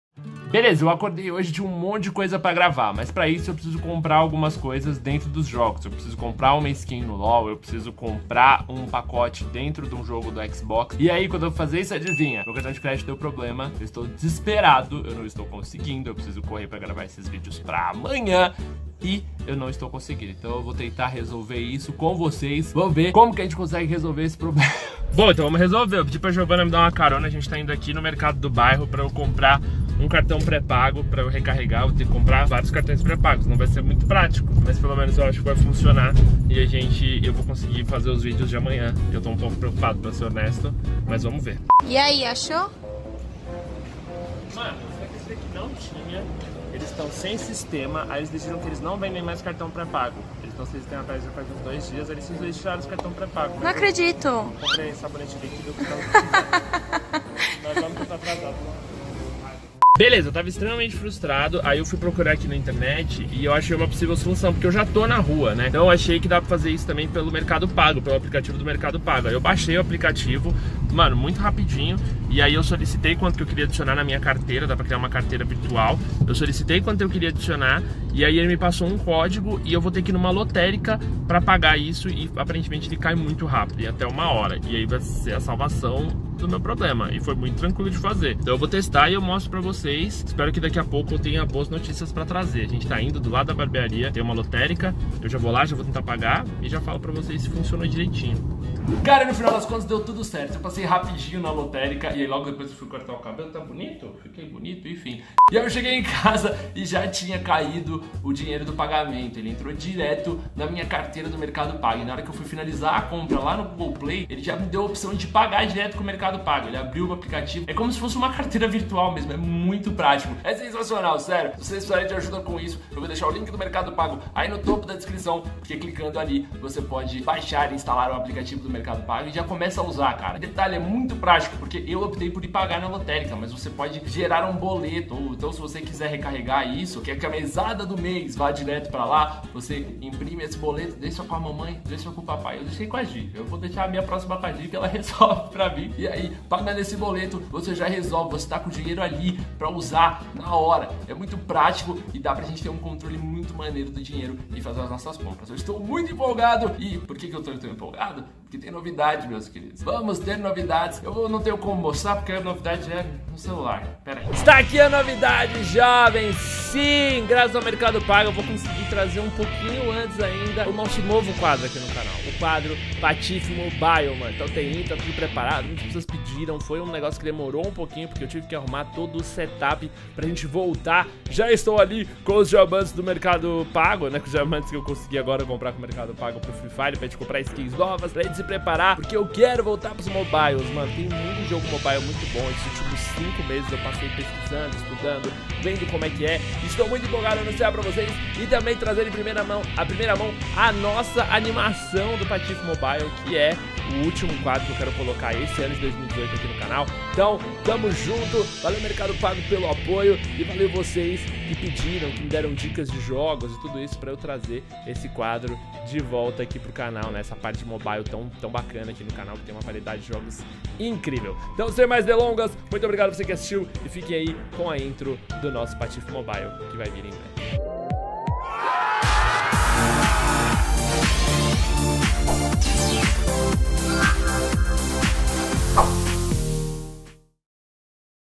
ESSE APP SALVOU O DIA!
Com tantas opções no mundo dos games, tudo que a gente mais quer é praticidade.  Se liga na dica: use o Mercado Pago nas suas compras na Google Play

Não esquece de avaliar, deixar seu comentário e compartilhar com a galera! Especialmente aquele seu amigo que sempre se complica 😂